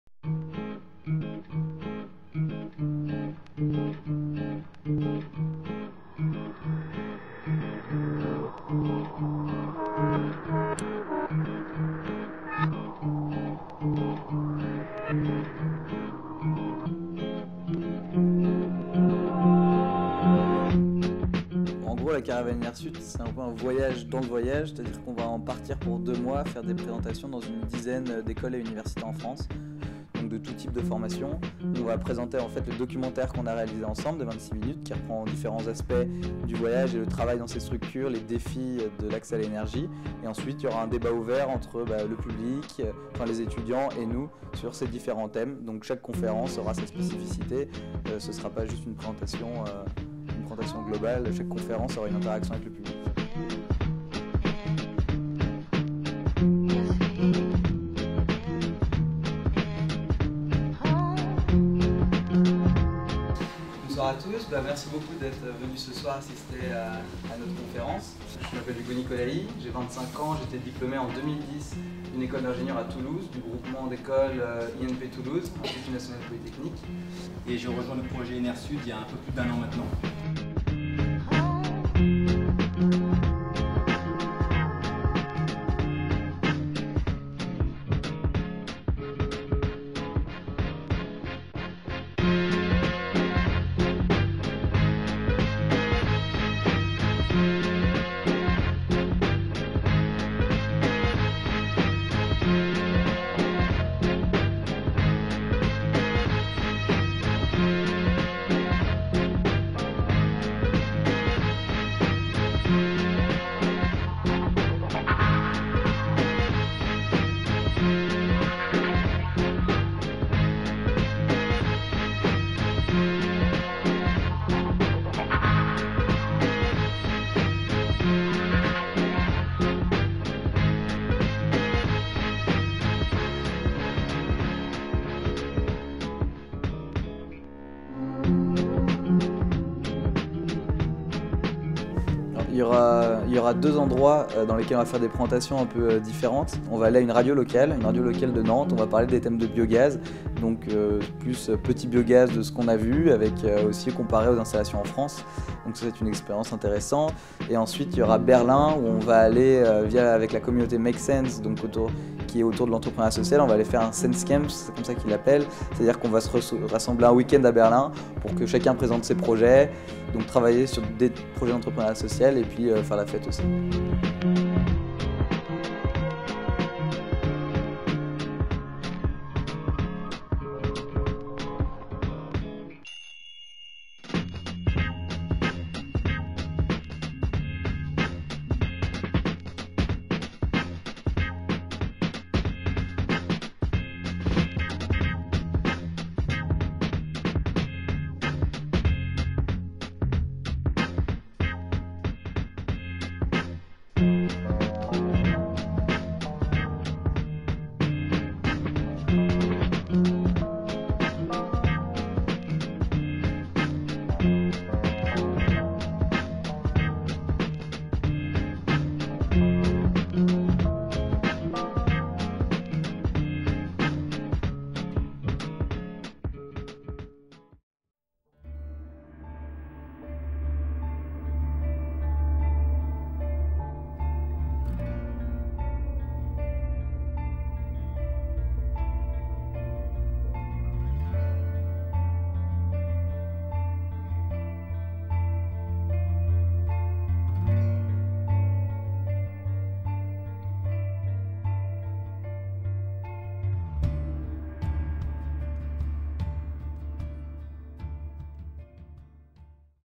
Caravane EnR'Sud
Tour de France des écoles et universités Janvier-Mars 2013: projection du film documentaire "l'Energie et de l'imagination" suivie d'un débat avec les étudiants sur accès à l'énergie, entrepreneuriat social et travail à l'étranger.